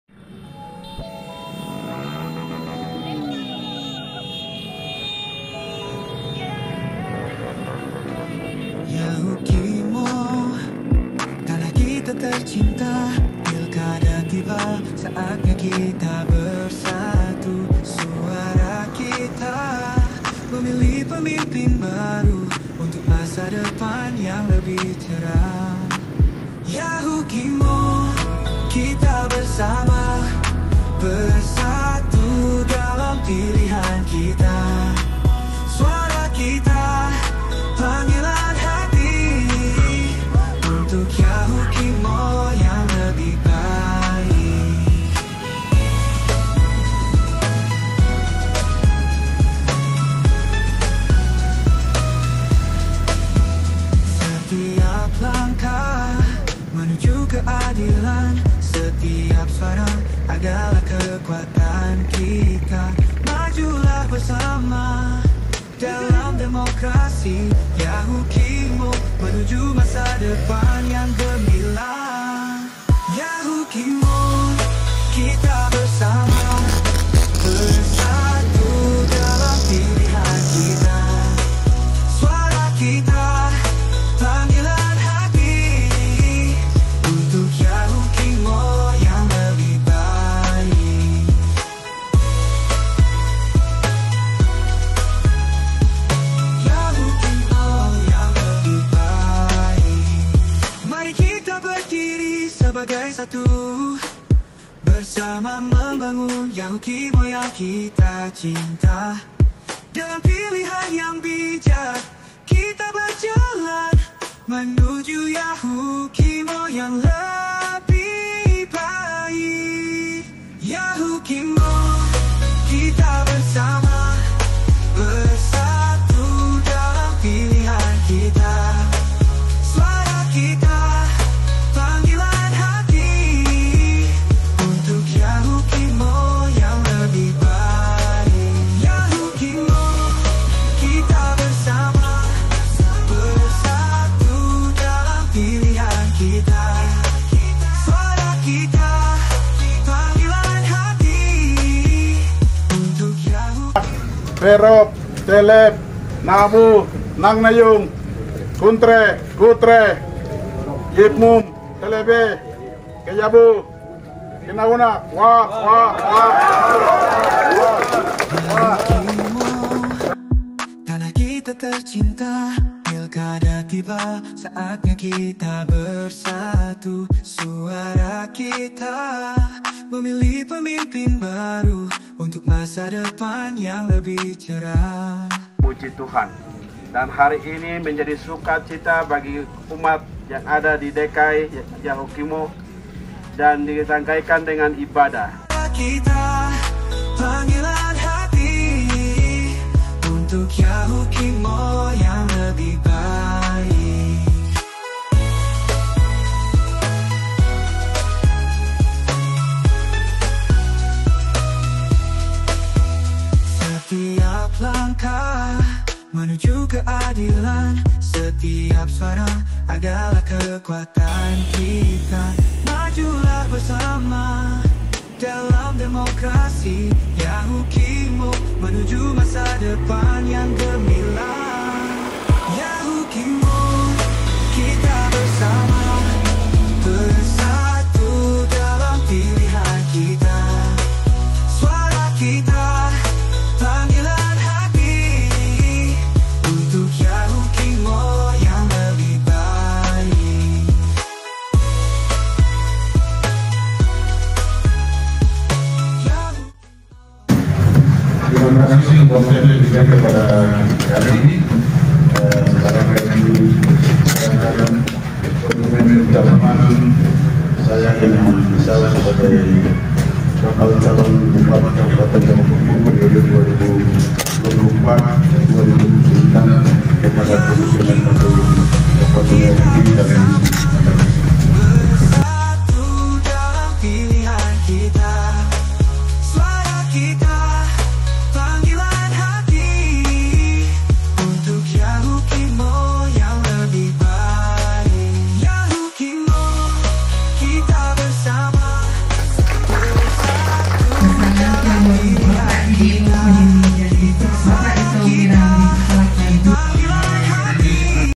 Didimus Yahuli dan Esau Miram mendaftar di KPU Kabupaten Yahukimo
Calon Bupati dan Wakil Bupati Yahukimo yang Daftar Pertama di kantor KPU Kabupaten Yahukimo adalah Pasangan Didimus Yahuli SH, dan Esau Miram, S.IP. Disingkat ( DYEM )